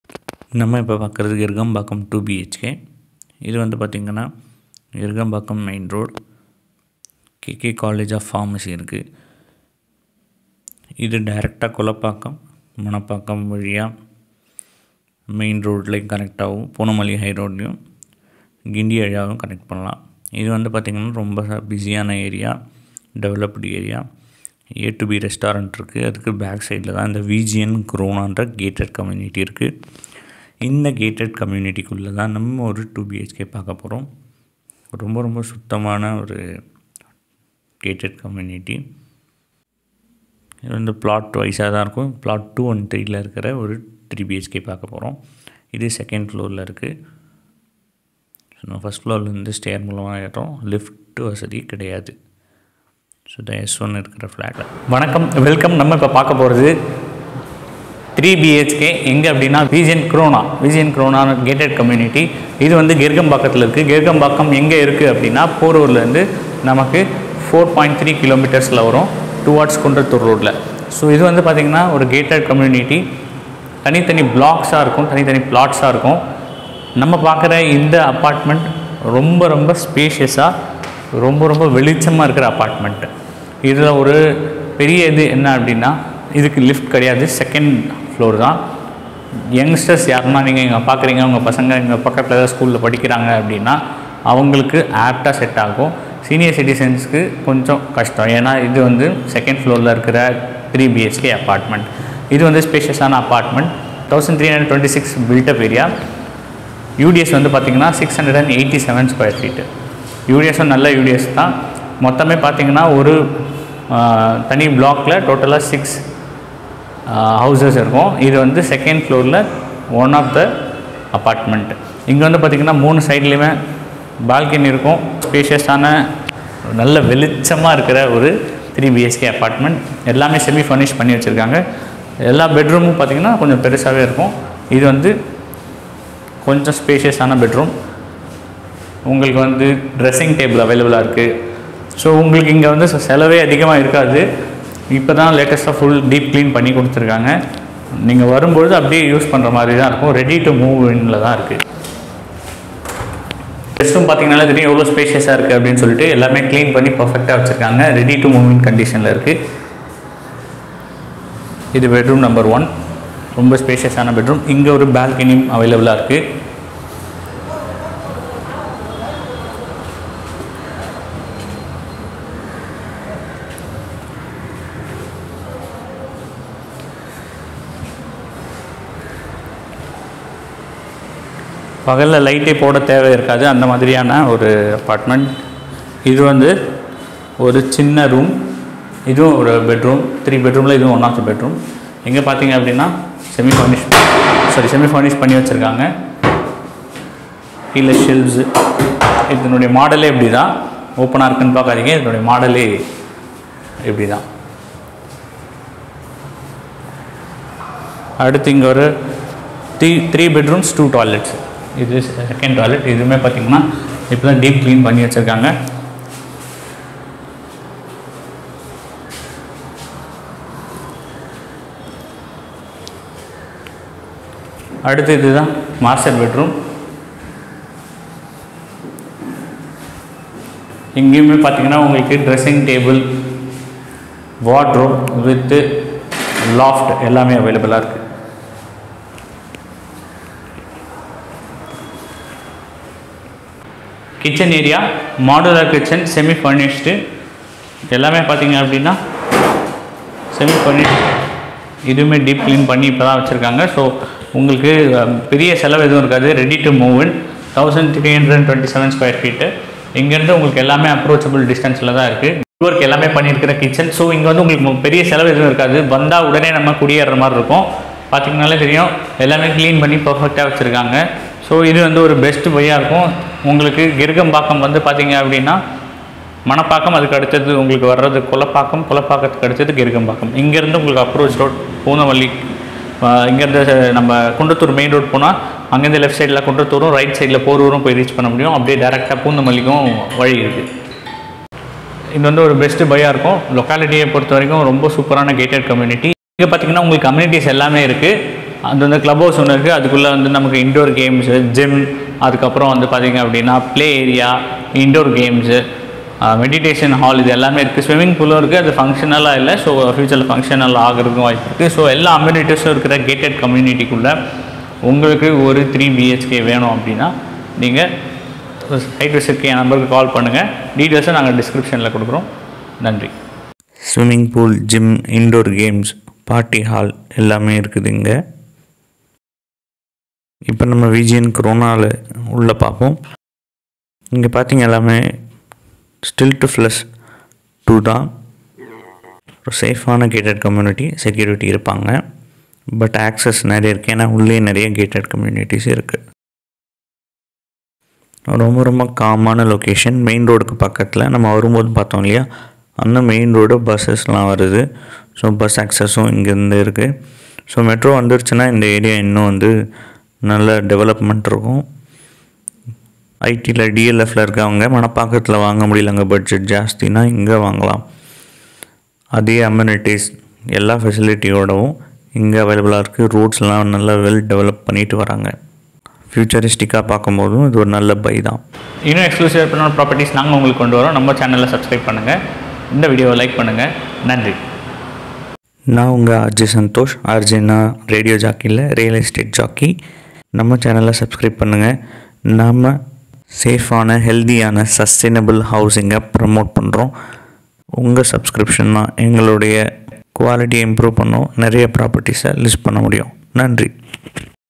Gated Community apartments in chennai with all amenities VGN KRONA near Porur at Gerugambakkam
Gated community apartments in chennai Gerugambakkam, Porur with all amenities

3 BHK For Sale @ Gerugambakkam, Near Porur

Second floor
9 years
65 Lakhs
No Lift
1 Covered Car Park
Swimming Pool
Gym
Party Hall
Garden Area

Chapters:
00:00 - Locality Tour
00:42 - Flat Tour
05:23 - VGN Krona Amenities and Gated Community tour

ReJ Santhosh Chennai (Real  Estate Jockey Chennai)
Realtor @Hanureddyrealty
Hanu Reddy Realty
International Real Estate Brokers